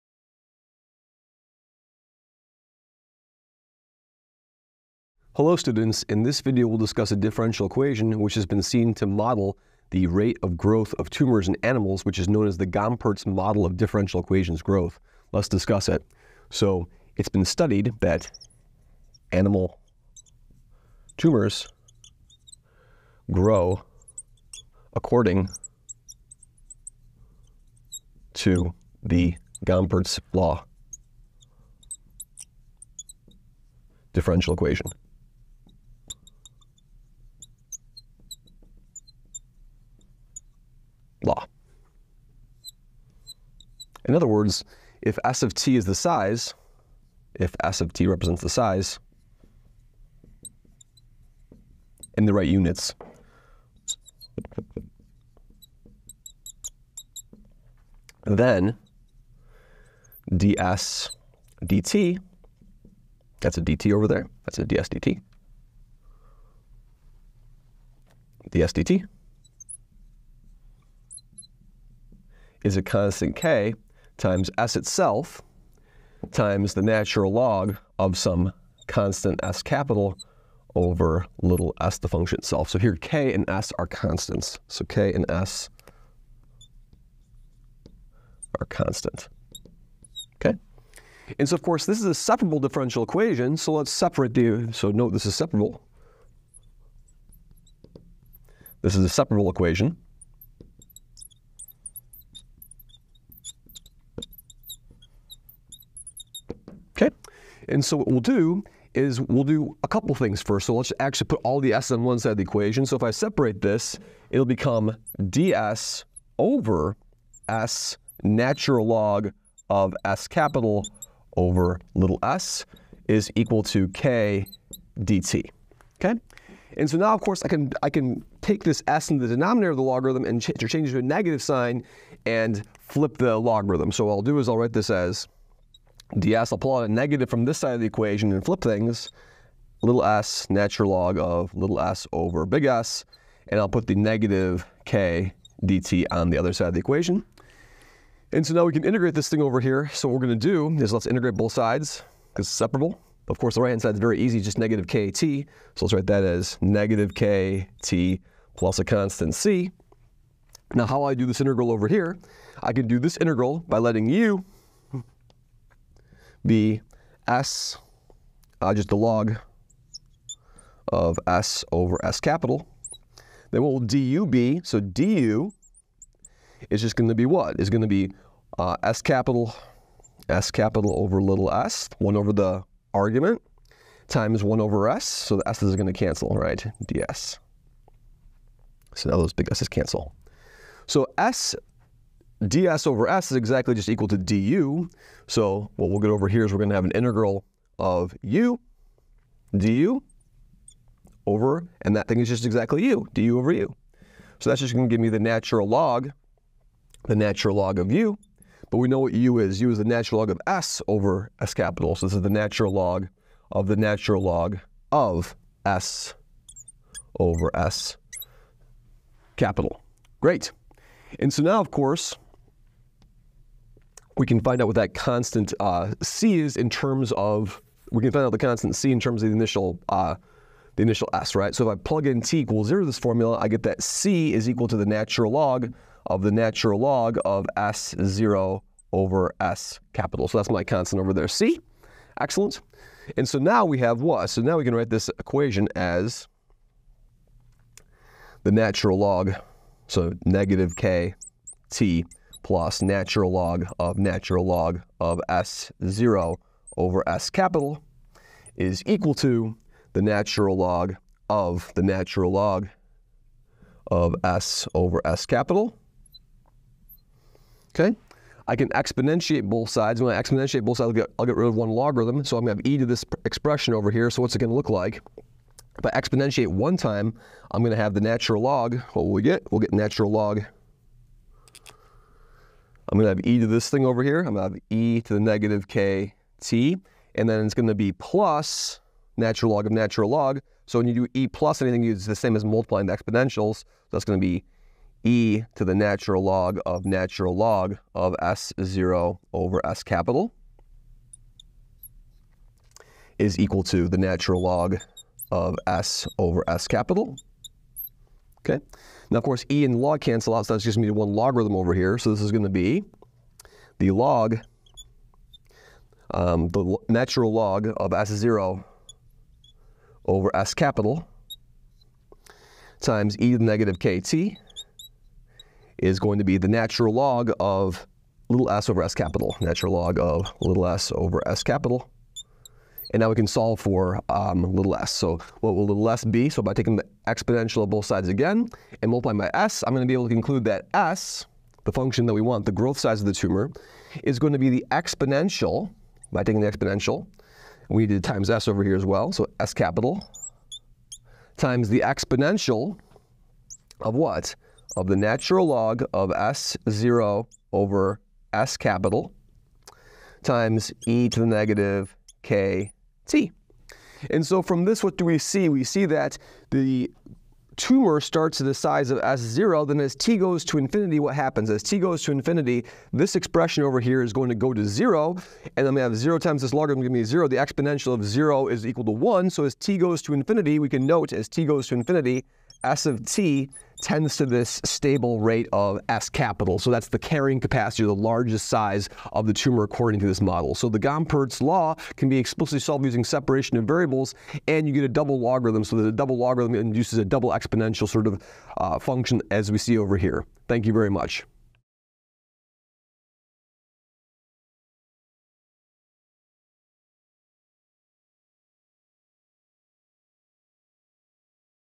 The Gompertz Differential Equation and Applications
We introduce the Gompertz Differential Equation. This differential equation is used in the modeling of tumor size in certain animals. It is a first order differential equation which can be explicitly solved. It's long time behavior is studied as well.